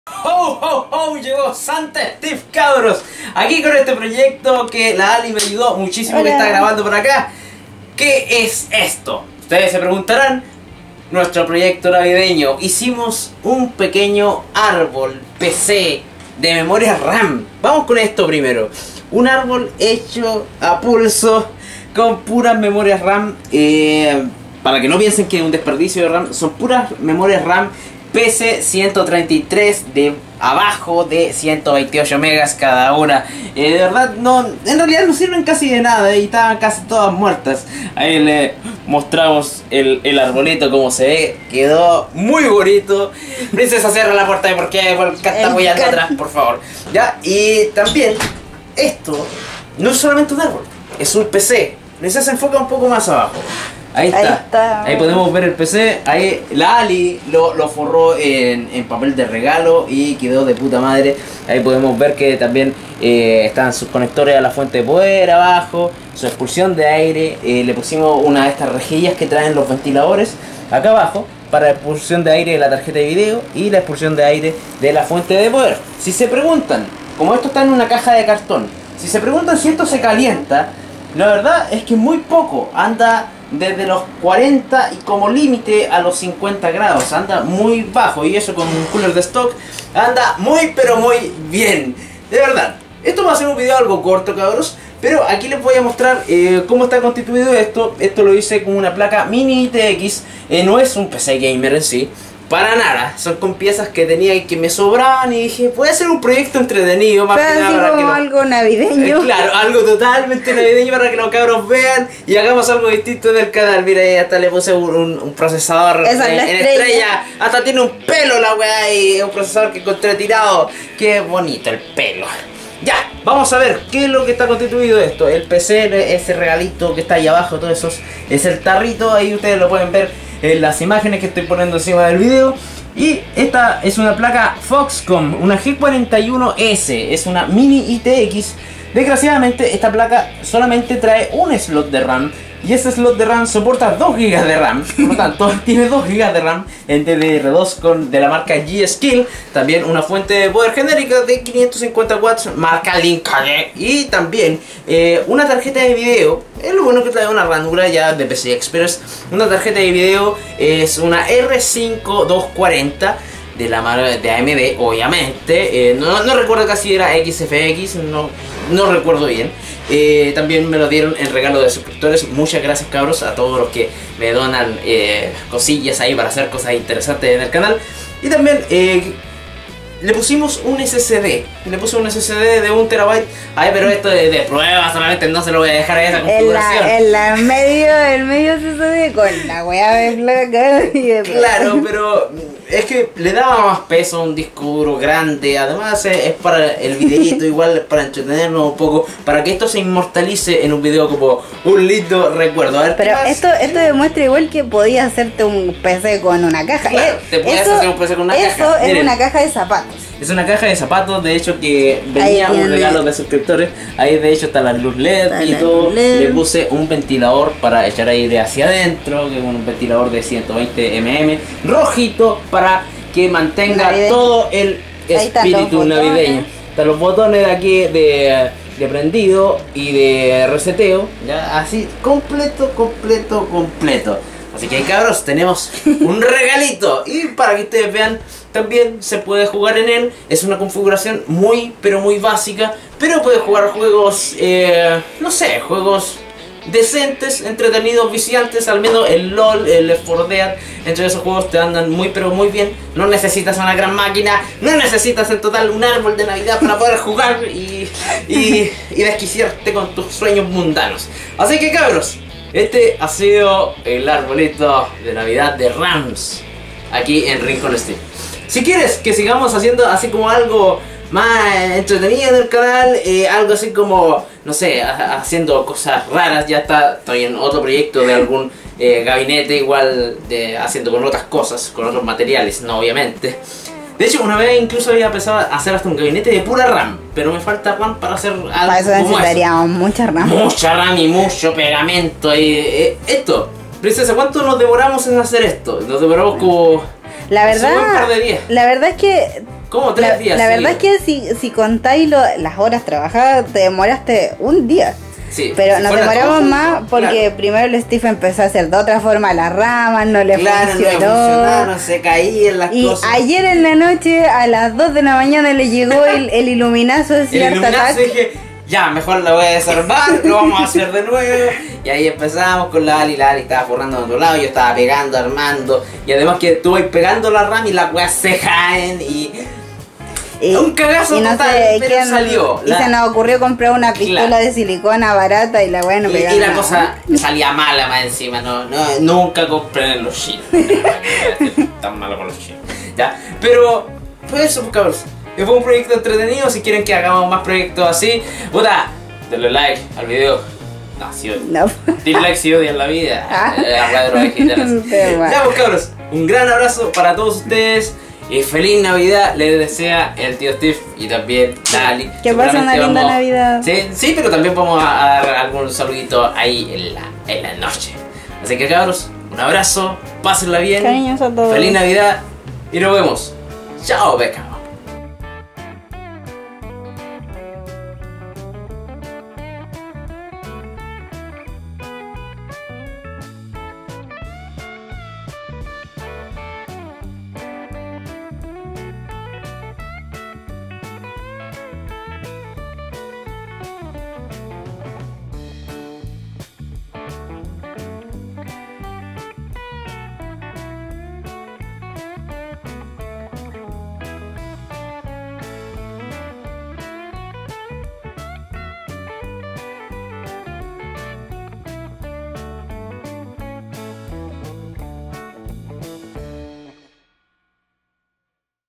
PC ARBOL DE NAVIDAD!?,ES UN ARBOL DE RAM GAMER! xD
Feliz navidad a todos"!!! :D

Quieres enviarnos una donacion? hazlo aqui!! :D

ask! (no estoy respondiendo preguntas de pc)